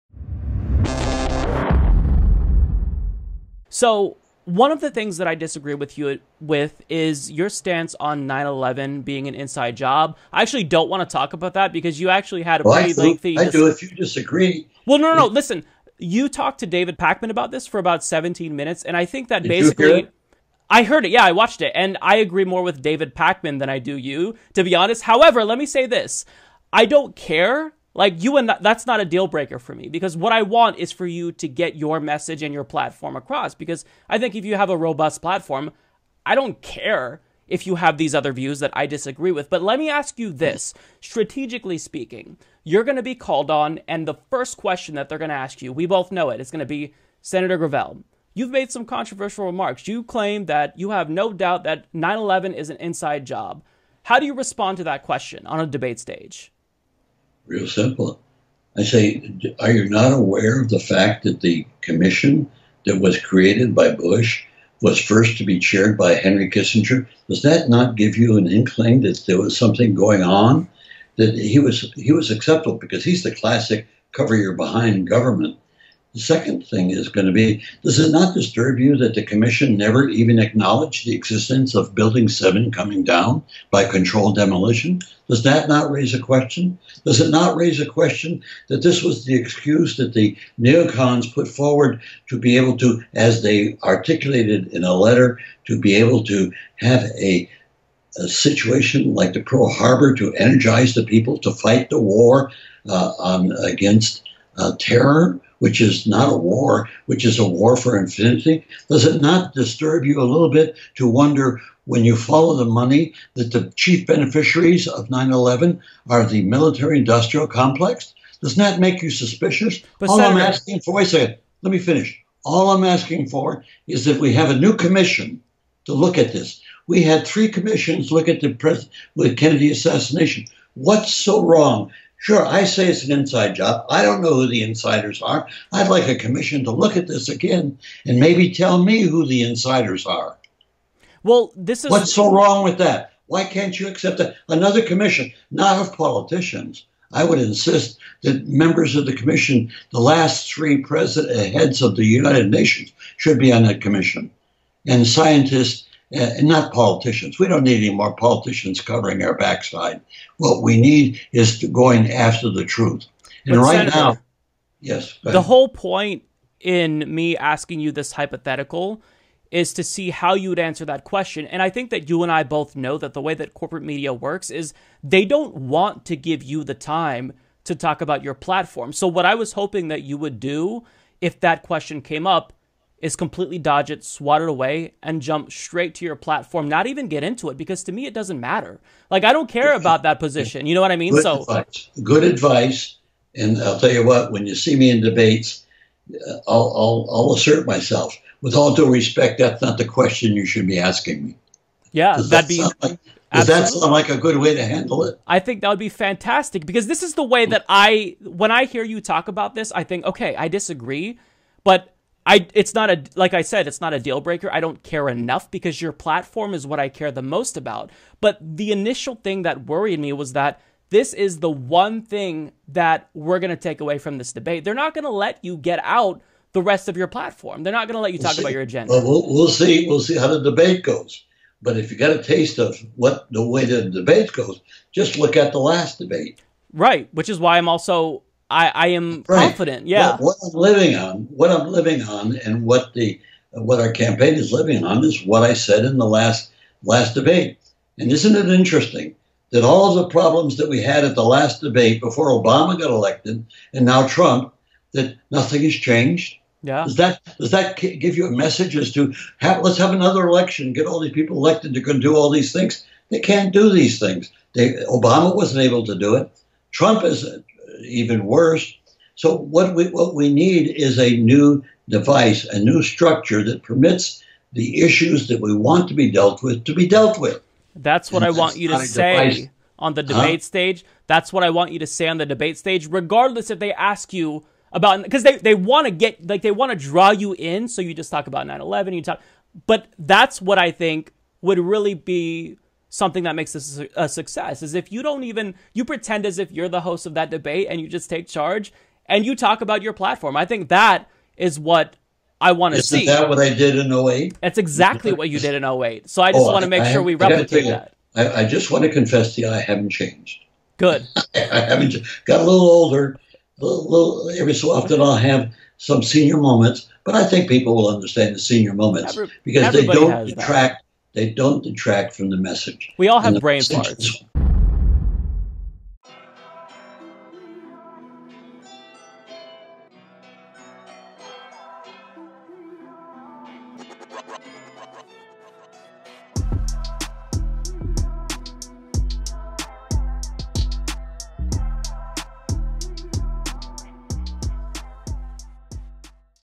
Will Mike Gravel Avoid Traps Set by Debate Moderators Over His 9/11 Views? | Interview Clip
Watch the Full Debate Here: [Link Coming Soon]